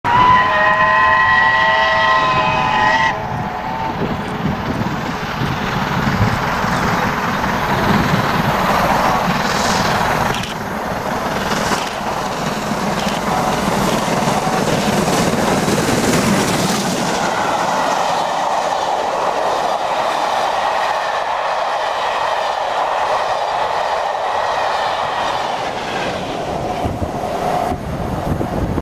Britannia on the English Riviera Express at Exminster. 02 July 2022
Britannia passing Exminster at speed with the English Riviera Express, in grim, wet weather on 02 July 2022